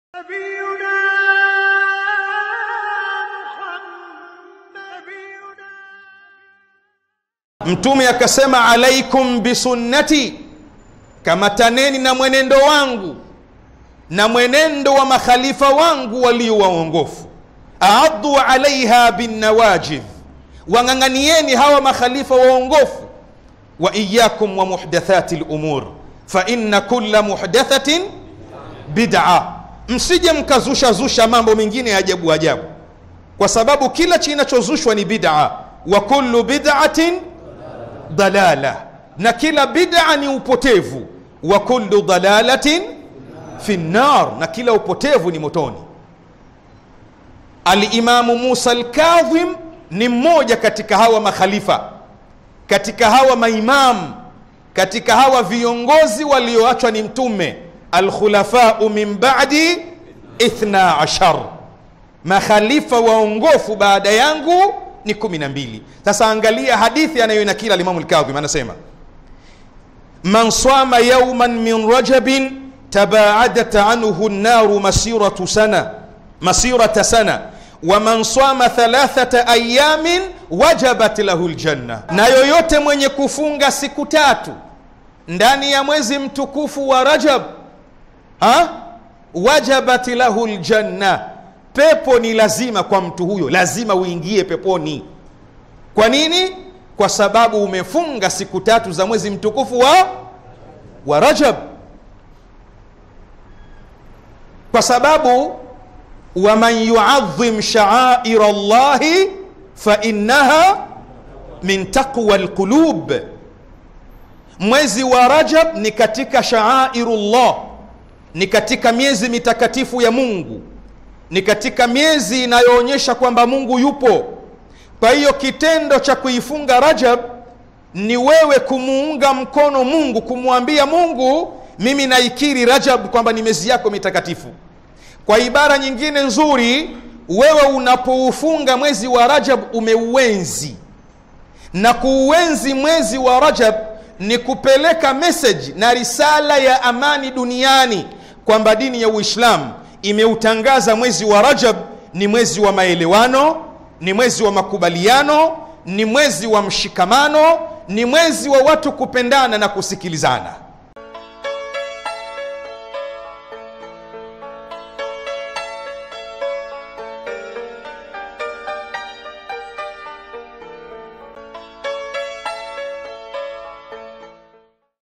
SWAUMU MWEZI RAJAB & FADHILA ZAKE
Mafunzo kutoka kwa makhalifa wa mtume juu swaumu na ibada za mwezi mtukufu wa Rajab .